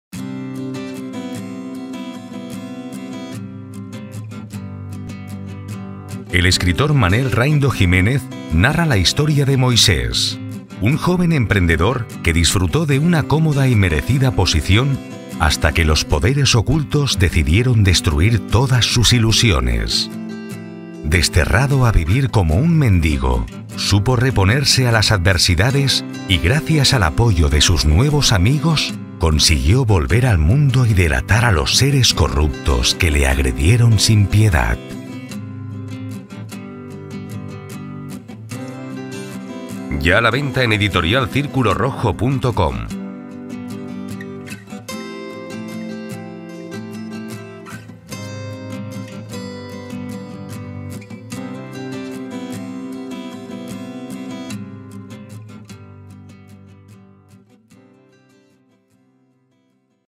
Lecciones de cartón (Booktrailer) - Editorial Círculo Rojo
Cuando se despertó rodeado de frío y cartones, no pudo evitar pensar lo peor. Moisés, un hombre joven con un futuro perfecto, sufre la ira de los que necesitan estar en la cumbre a costa del sufrimiento de los demás. La desesperación, no consiguió borrar su fuerza interior y gracias al cariño de quien aún lo regala sin vender su alma al diablo, consigue descubrir el camino que le permitirá salir del infierno. La justicia que un día le dio la espalda, acaba siendo su aliada y juntos descubrirán que un mundo limpio de maldad aún es posible. El amor y la amistad hicieron el resto.